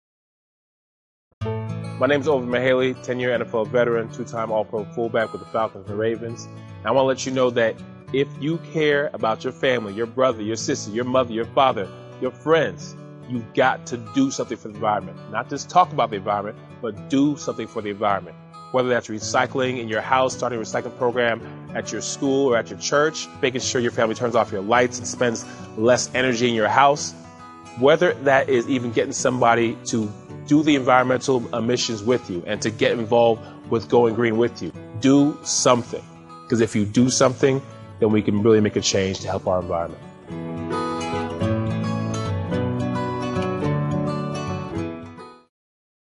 Football and The Environment!!!
Former football player Ovie Mughelli talks about what you can do to make a difference for the environment.